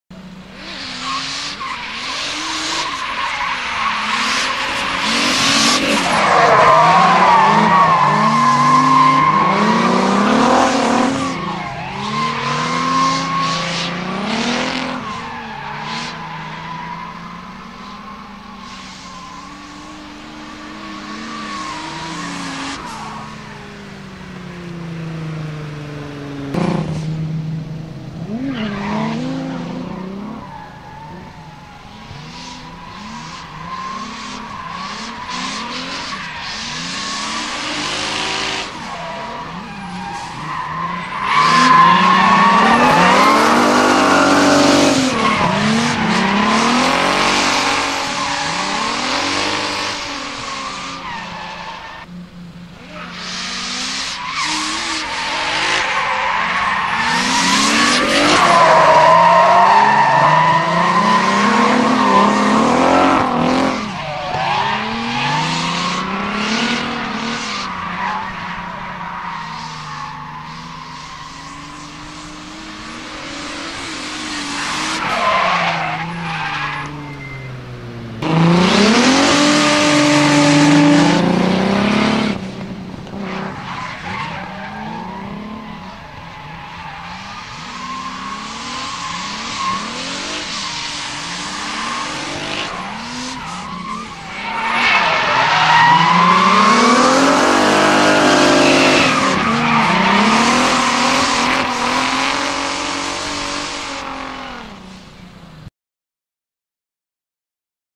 Drift Iceland 2016! Twin Drift BMW 330i Turbo & BMW 518i
Drift Iceland 2016!
Twin Drift
Drivers: Aron Jarl Hillers - BMW 330i Turbo (red) & Þórir Örn Eyjólfsson - BMW 518i